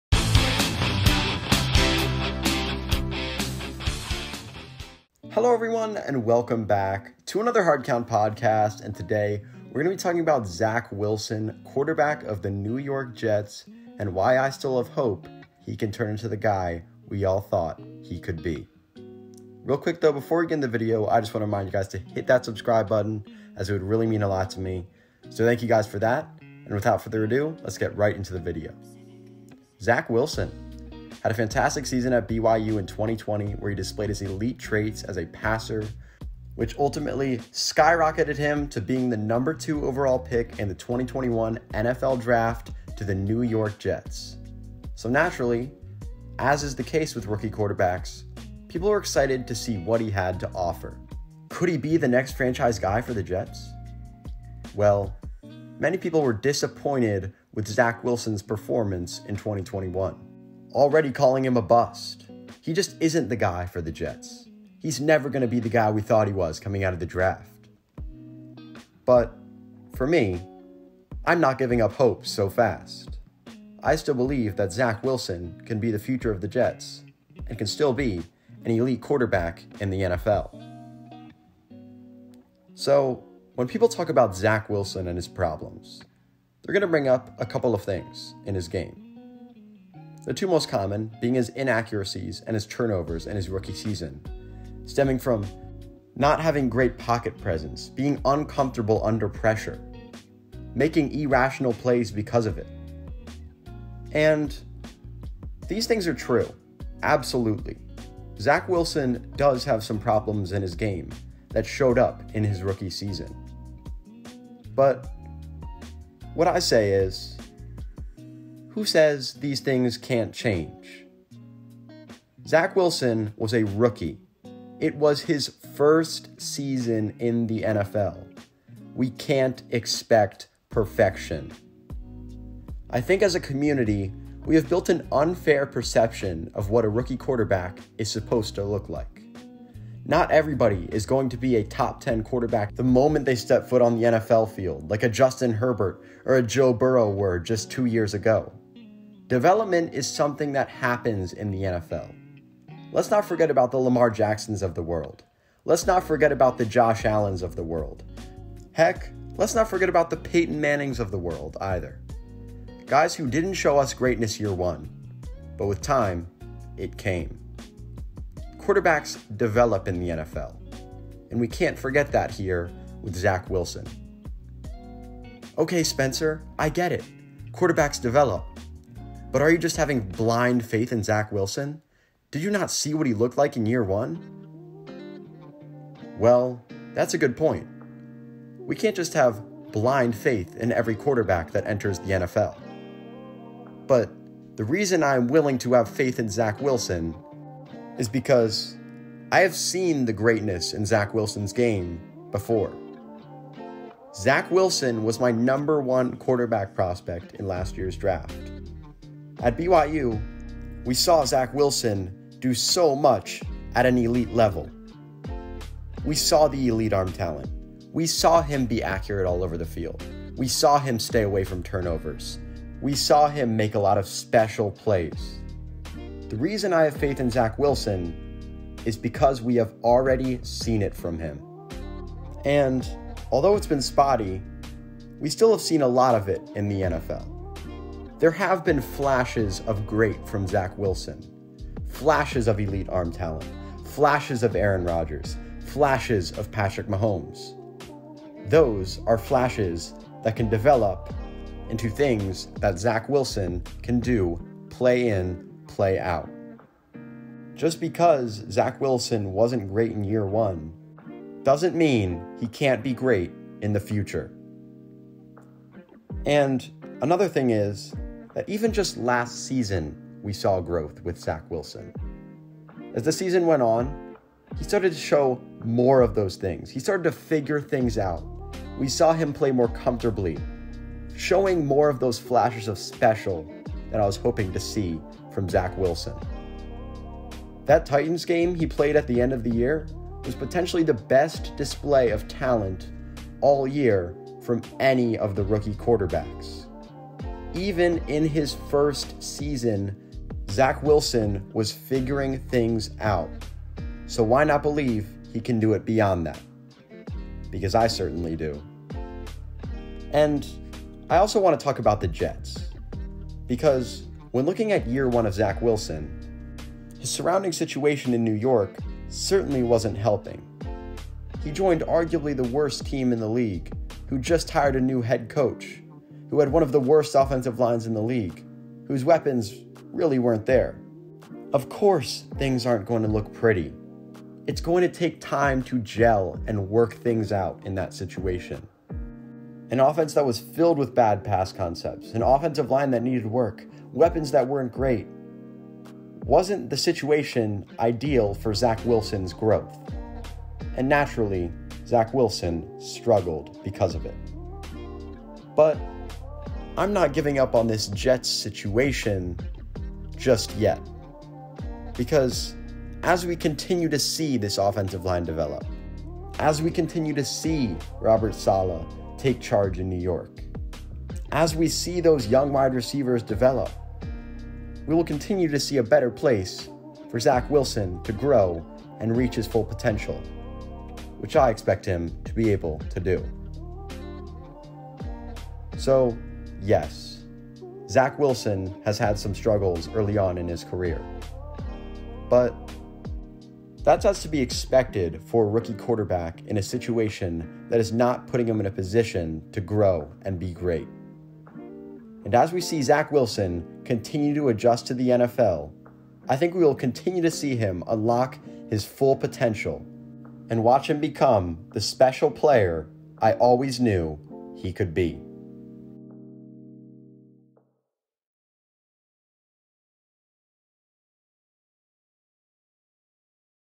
Let's not give up on Zach Wilson just yet
In this video I talk about Zach Wilson and why he can still become the elite quarterback we thought he could be coming out of the draft. I talk about big names like Zach Wilson, Justin Herbert, Joe Burrow, Lamar Jackson, Josh Allen, Robert Saleh, Elijah Moore, and more.